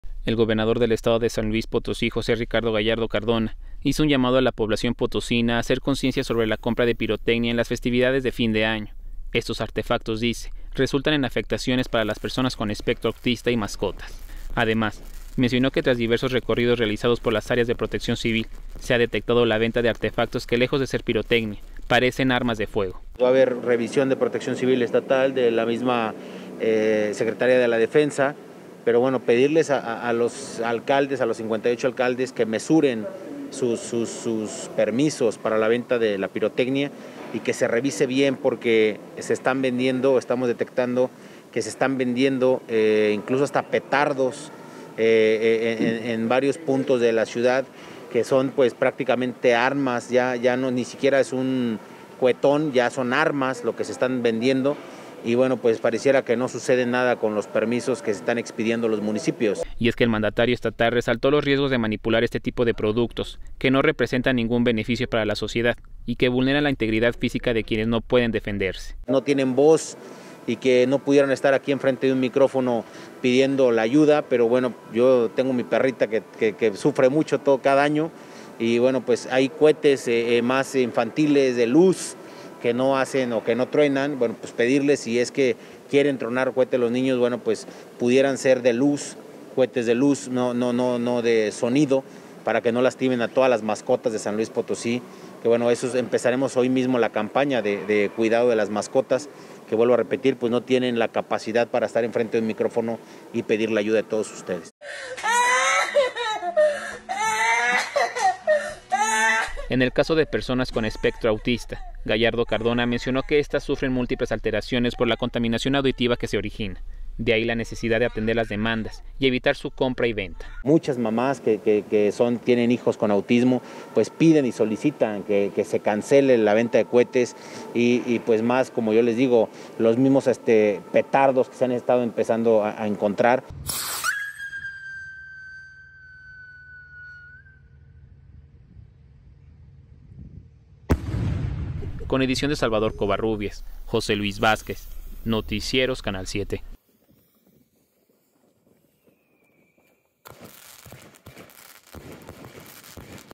Autoridades advierten que en temporada decembrina hasta petardos se venden en diferentes puntos.
SLP.- El gobernador del estado de San Luis Potosí, José Ricardo Gallardo Cardona, hizo un llamado a la población potosina a hacer conciencia sobre la compra de pirotecnia en las festividades de este fin de año, estos artefactos dice, resultan en afectaciones para las personas con espectro autista y mascotas.

Además, mencionó que tras diversos recorridos realizados por las áreas de protección civil, se ha detectado la venta de artefactos que lejos de ser pirotecnia parecen armas de fuego.

“Va a haber revisión de parte de Protección Civil Estatal, Secretaría de la Defensa, pero bueno, pedirle a los alcaldes, a los 58 alcaldes que mesuren sus permisos para la venta de pirotecnia y que se revise bien porque se están vendiendo incluso hasta petardos en varios puntos de la ciudad que son armas, ya no es un cuetón, son más bien armas, y parece que no sucede nada con los permisos que están expidiendo los municipios”, dijo.

Y es que el mandatario estatal resaltó los riesgos de manipular este tipo de productos que no representan ningún beneficio para la sociedad y que vulneran la integridad fisica de quienes no pueden defenderse.

“No tienen voz y no pueden pedir frente a un micrófono la ayuda, pero yo tengo mi perrita que sufre mucho cada año, y hay cuetes más infantiles de luz que no truenan, para que no lastimen a todos los animales de San Luis Potosí, que no tienen la capacidad de pedir la ayuda”, agregó.

En el caso de las personas con espectro autista, Gallardo Cardona mencionó que esfas sufren múltiples alteraciones por la contaminación auditiva que se origina, dd ahí la necesidad de atender las demandas y evita su compra y venta.

“Nos están haciendo llegar a través del DIF Estatal, muchas mamá con niños con espectro autista, piden y solicitan que se cancele la venta de cuetes, y más los petardos que se han llegado a encontrar”, terminó.